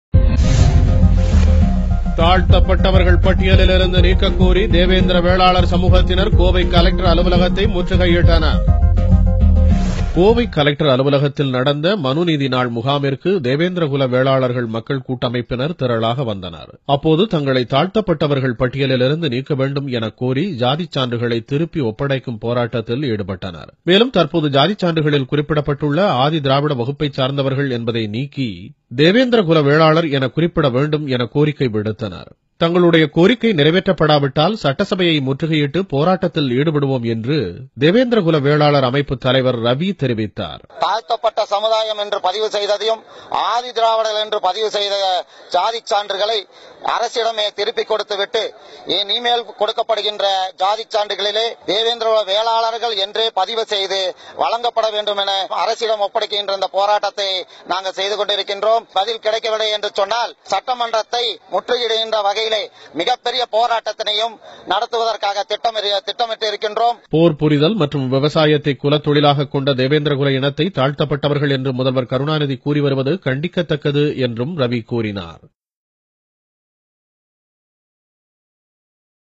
Coimbatore devendrakula vellalar
1.Devendrakula vellalar wants their name to be changed from Pallars to Devendrakula vellalar
2.Devendrakula vellalar people wants to remove their caste scheduled list